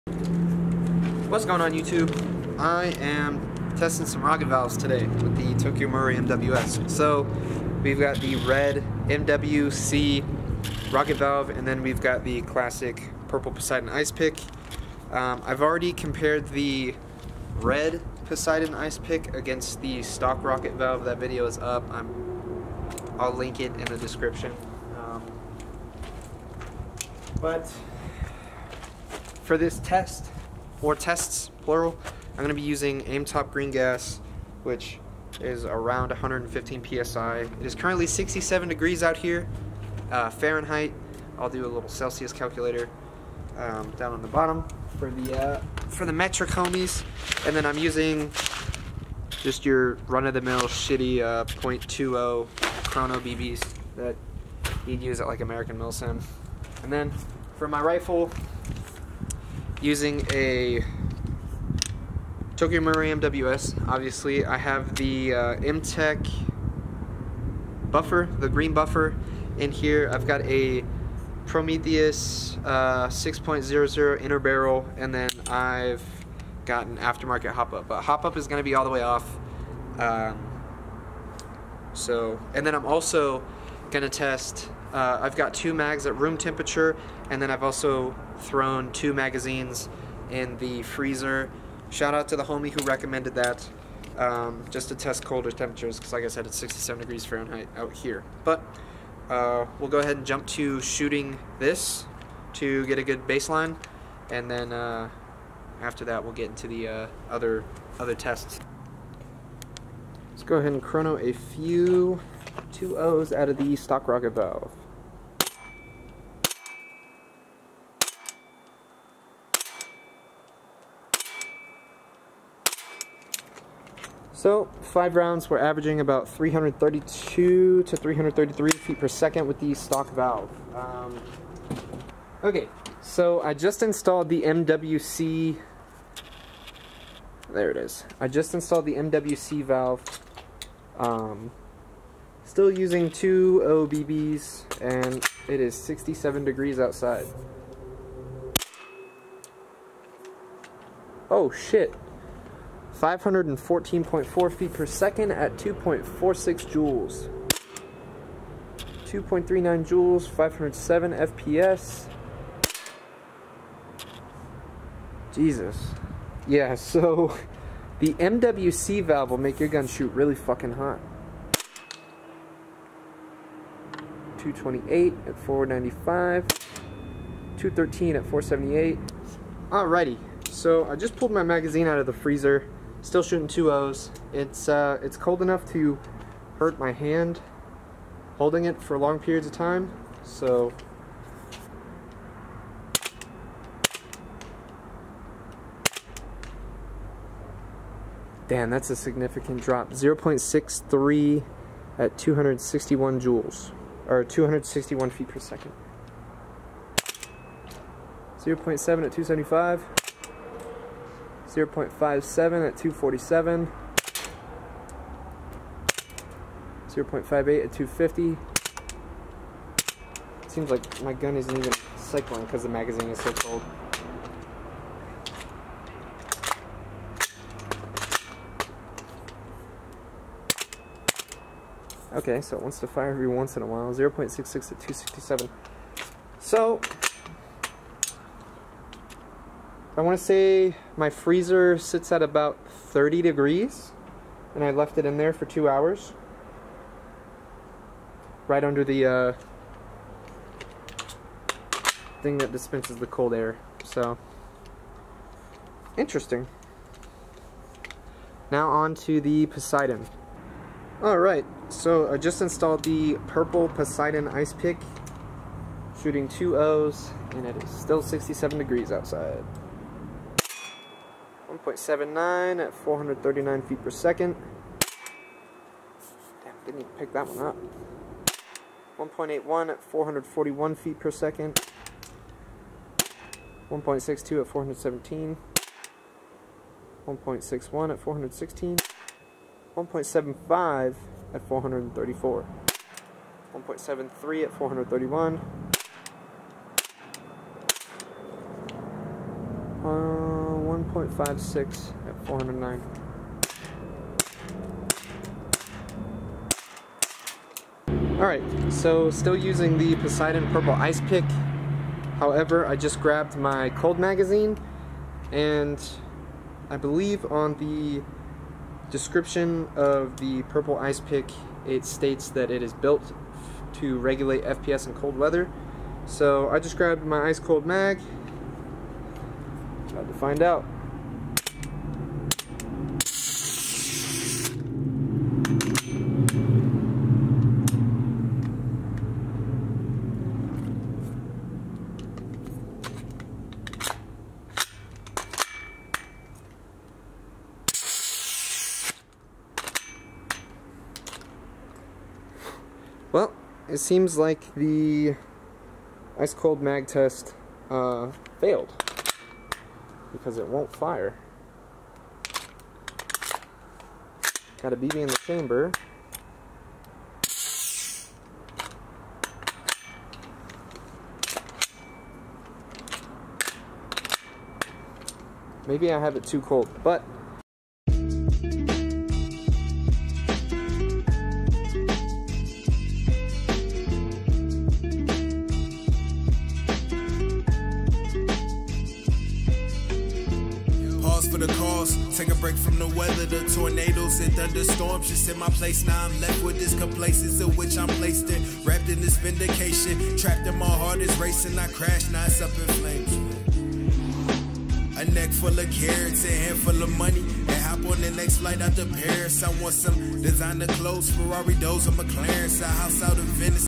Technical Discussion: MWC & Purple Ice Pick testing
Testing the MWC and the Purple Poseidon Ice Pick for science! I gotta get my flow down instead of saying uh or okay too many damn times.

Inner barrel length is 370mm which is why it was shooting hot, shouldve added that into the video. My b.